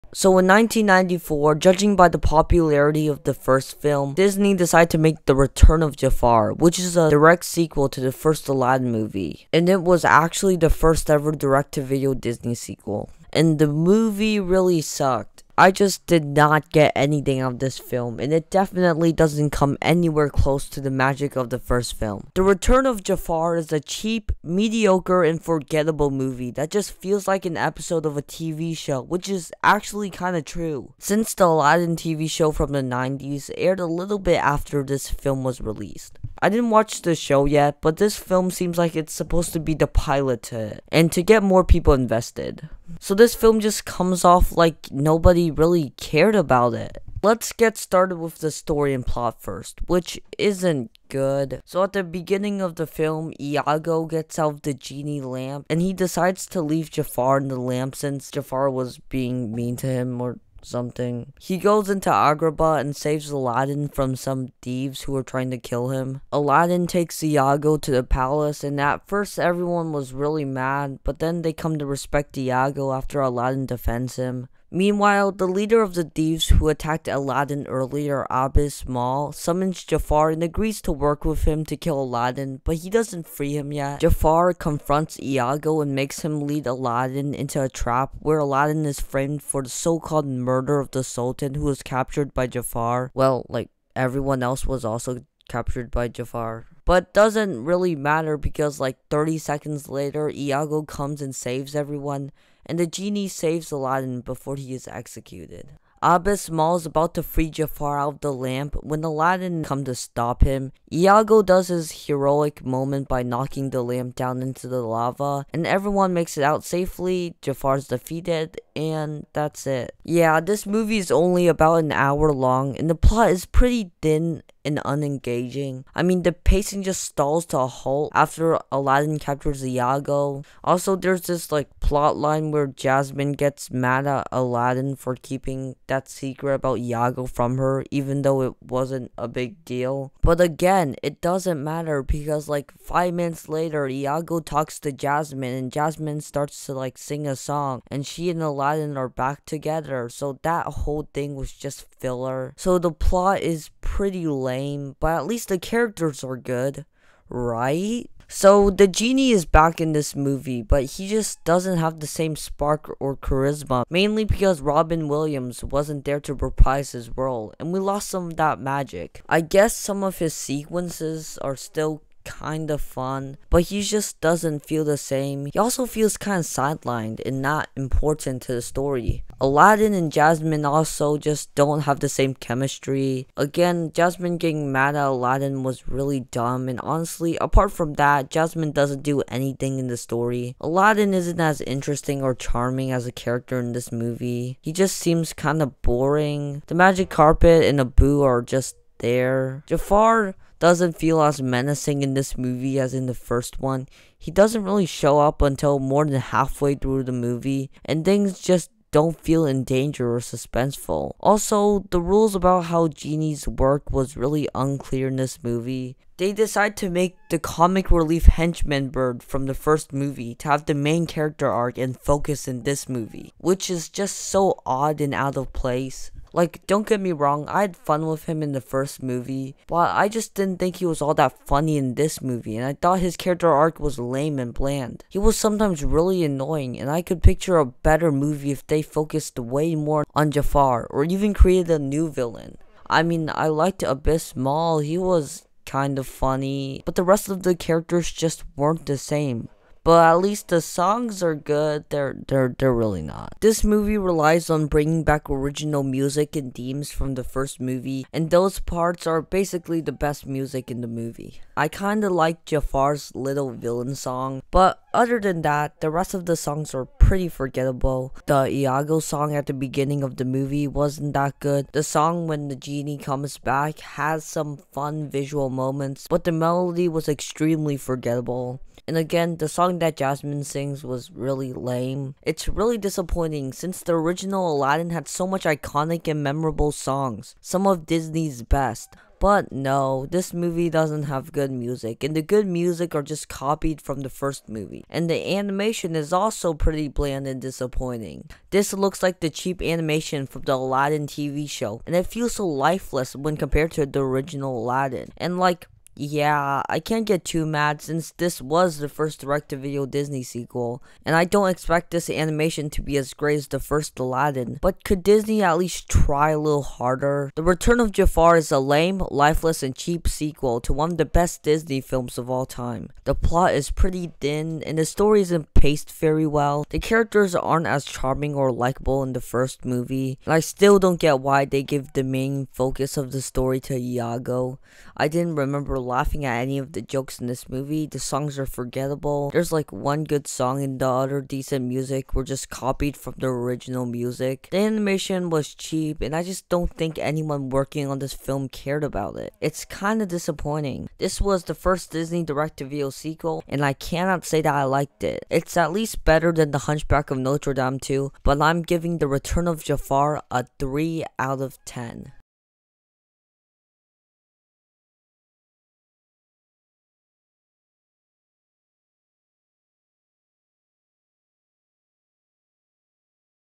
Aladdin: The Return of Jafar is Incredibly Meh
Aladdin: The Return of Jafar Review
Disney Direct to Video Sequels Reviewed Ep.2